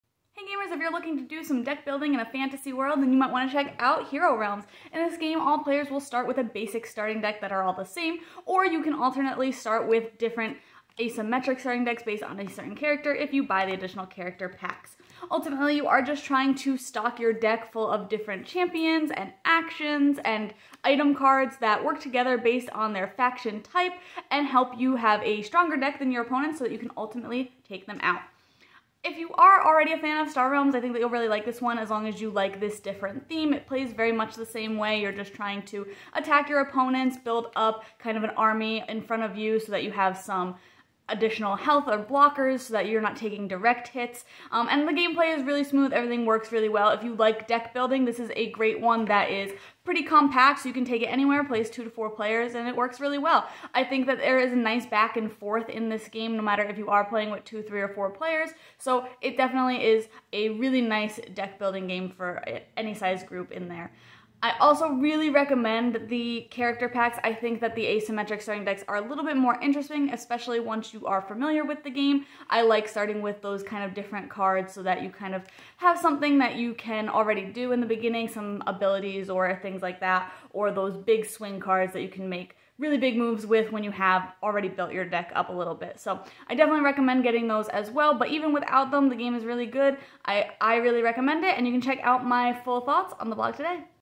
Hero Realms Quick Review
If you like deck building in a fantasy theme, with plenty of PvP interaction, check this one out!

*I was provided a copy of this game to do this review*